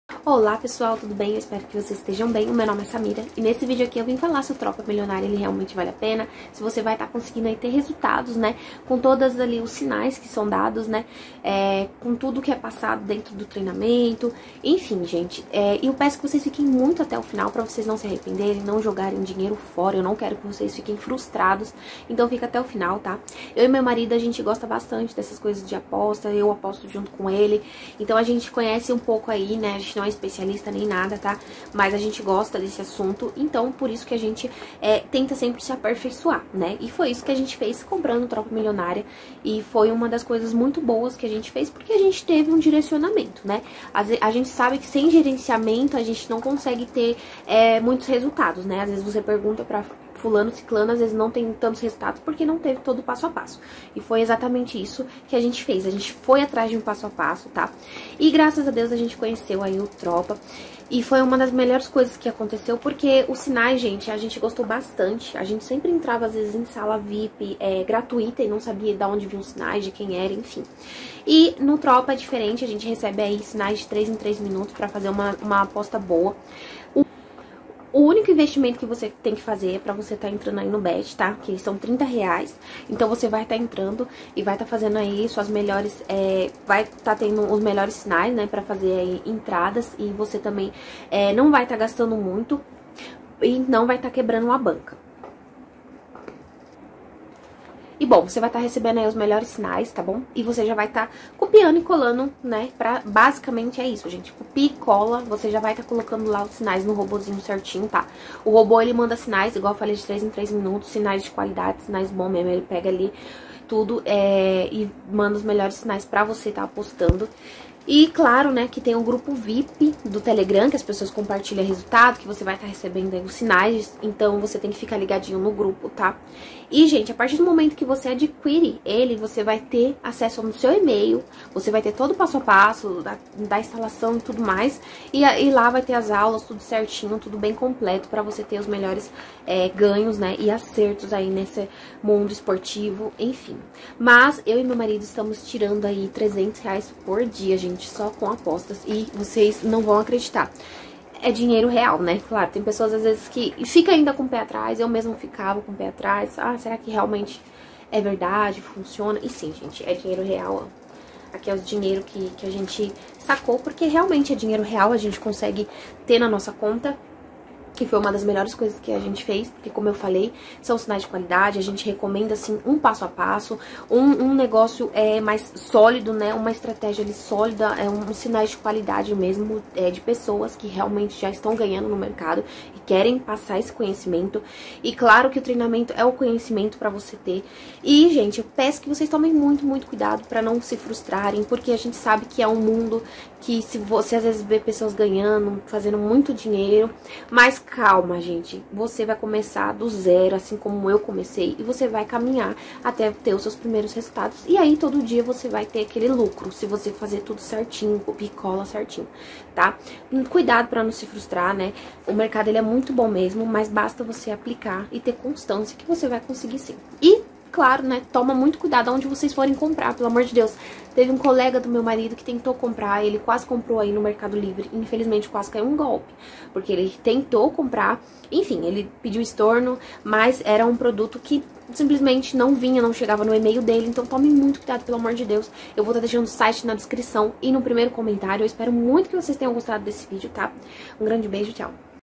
Tropa Milionária - Tropa Milionária funciona ? Tropa Milionária vale apena ? Tropa Milionária
Olá pessoal tudo bem ? bom venho falar sobre o curso tropa milionária
Um treinamento bem completo e bem do básico ao avançado como falei no vídeo meu marido e eu gostamos muito desse tipo jogos e melhor ainda quando se ganhar dinheiro.
você não precisa entender muito, não precisa saber absolutamente nada sobre fazer análises complicadas e nem nada do tipo...
Tropa milionária funciona ?
Sim o treinamento tem grupo de suporte, sinais de 3 em 3 minutos. valeu super apena.